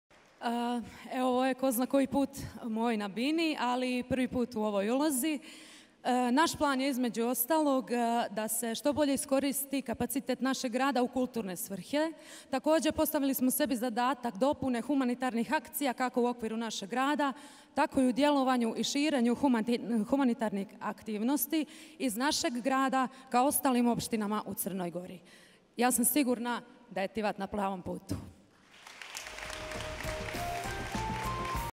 Nina Petković na konvenciji liste DPS Evropski Tivat 14. aprila 2016. godine.
Nina Petković, kandidatkinja  na listi DPS Evropski Tivat  na konvenciji 14. aprila 2016. godine.

Konvencija liste DPS Evropski Tivat održana je uoči lokalnih izbora u Tivtu zakazanih za 17. april 2016. godine